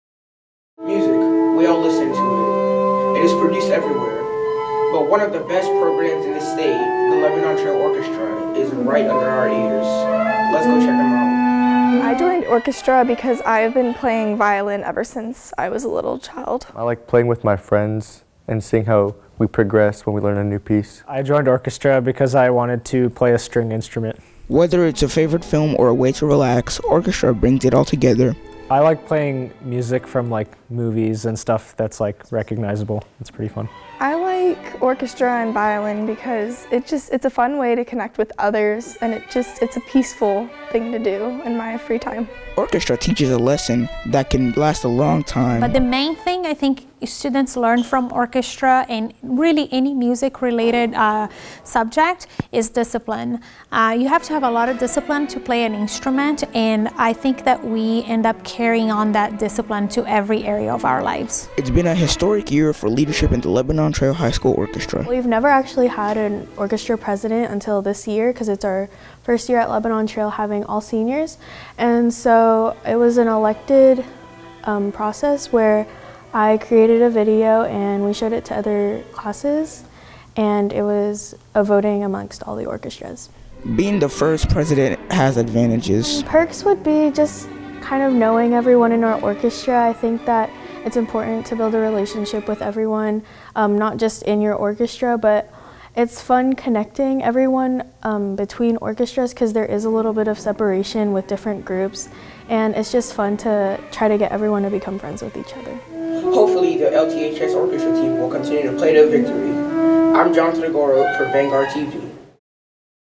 LTHS Orchestra
VanguardTV's Jonathan Oghoro gets in tune with the Lebanon Trail High School orchestra and their year of first-time happenings.